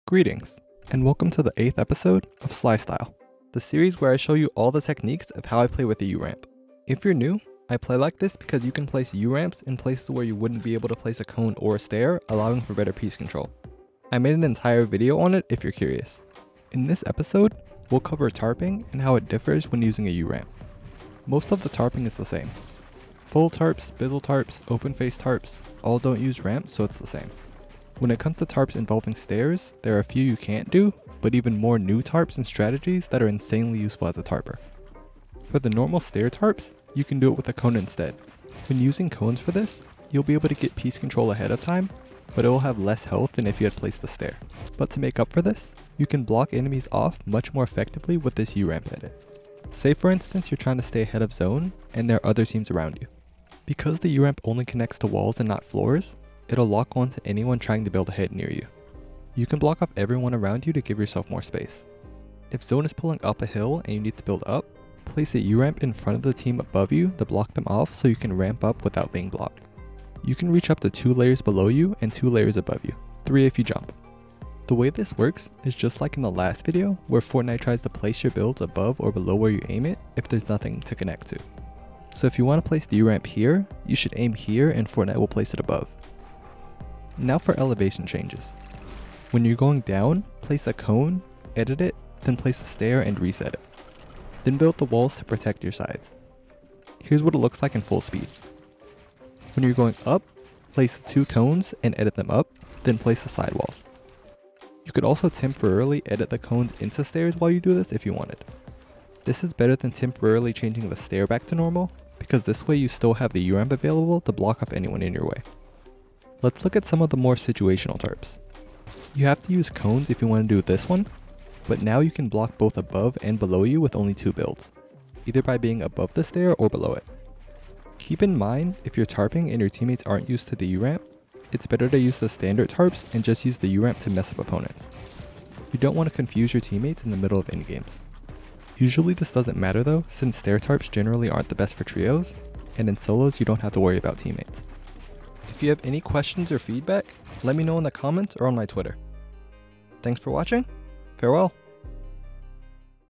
Sly Style: Tarping ⭐ U Ramp Playstyle ⭐ Pre-Edits: Episode 8
In this episode of Sly Style, We go over Tarping.

This series covers every single technique for how I play with a U-Ramp. From 90s, to Super Jumps, to Tarping, everything is covered.